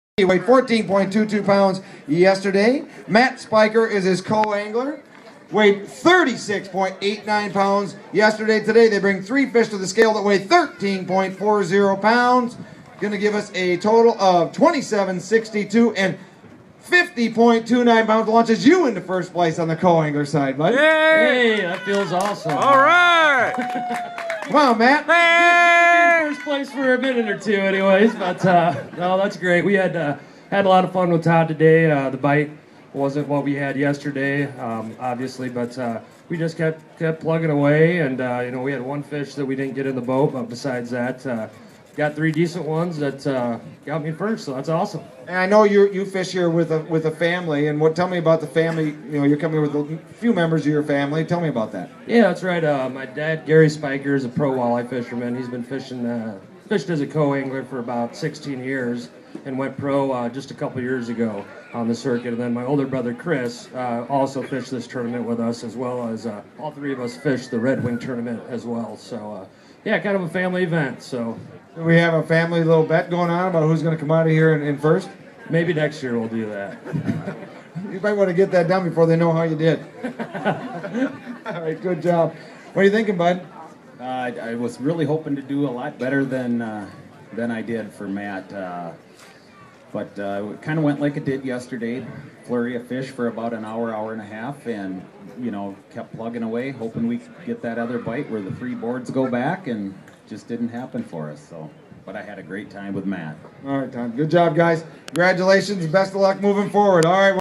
NWT Weigh in Day 2 with Matt Speicher
Matt weighs in at day two of the National Walleye Tour tournament at Green Bay.